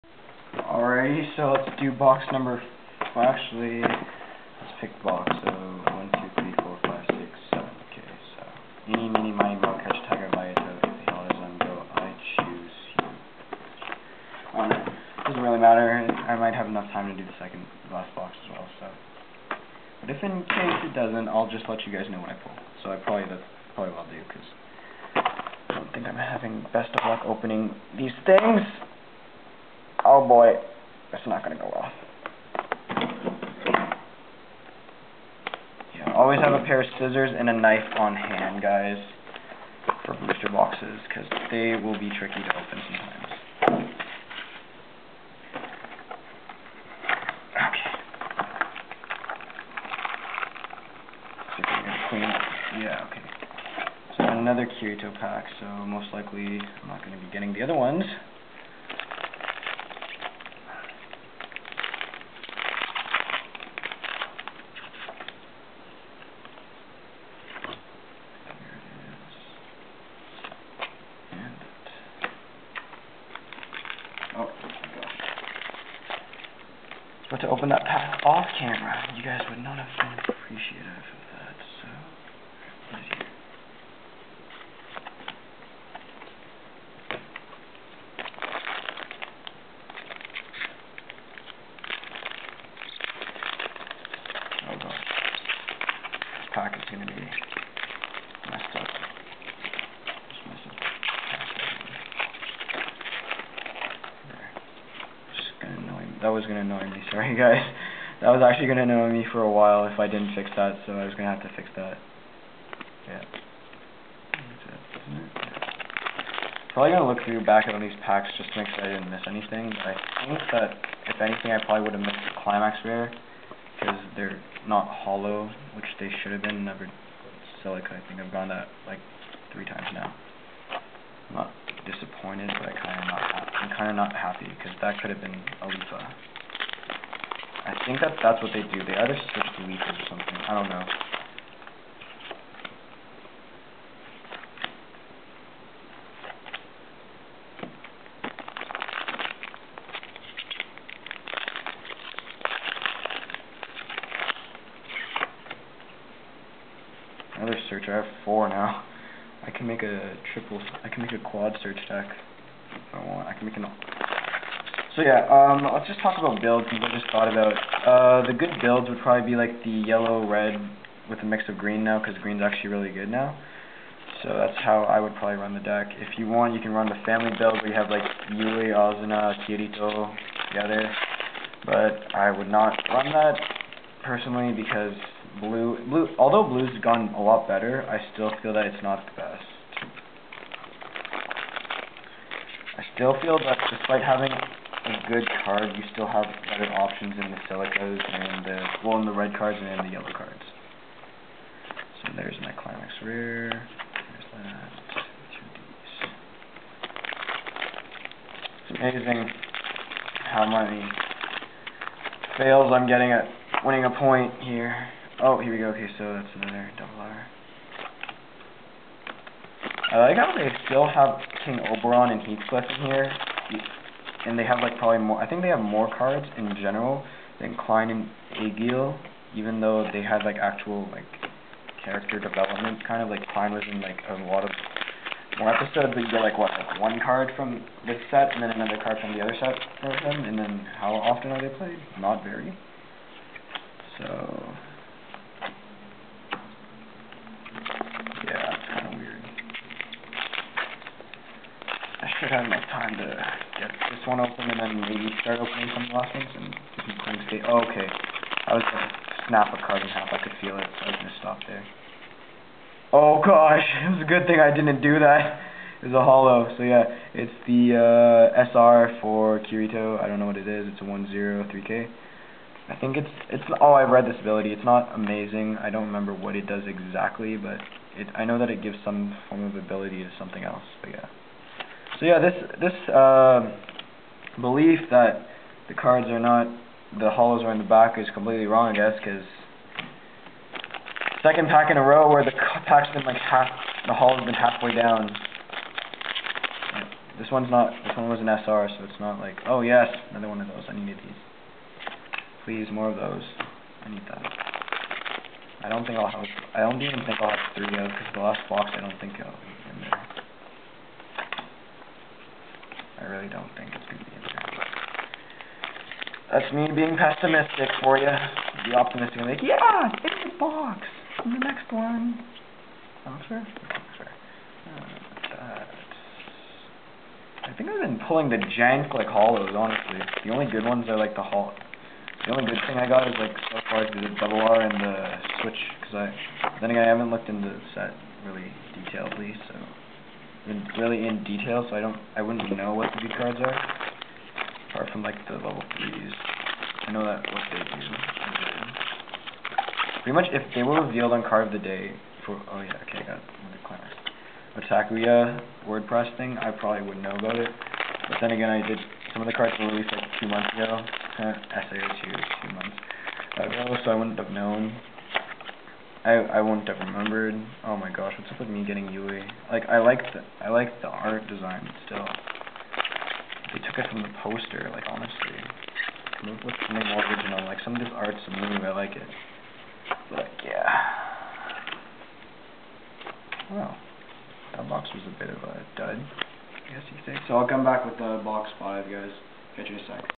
Sword Art Online 2 Booster Box Opening 3/4
Opening up my SAO Vol. 2 Boosters for you guys. PM us on either Youtube or Facebook, for any wants/trades on anything.